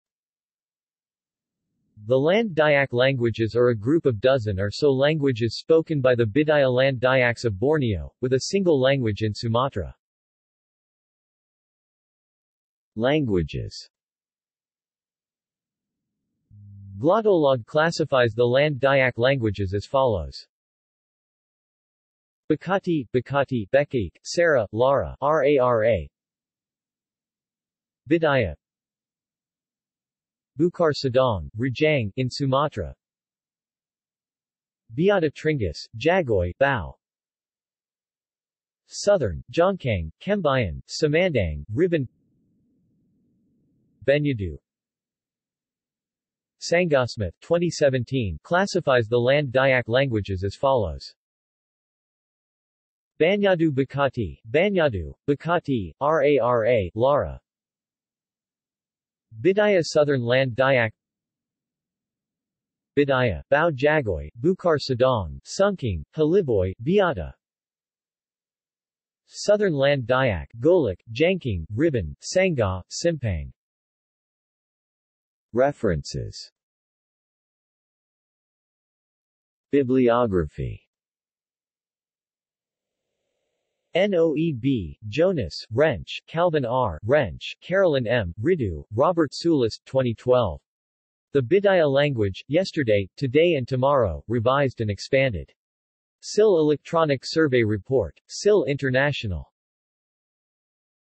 Land Dayak languages | Wikipedia audio article
This is an audio version of the Wikipedia Article:
Land Dayak languages

SUMMARY
The Land Dayak languages are a group of dozen or so languages spoken by the Bidayuh Land Dayaks of Borneo, with a single language in Sumatra.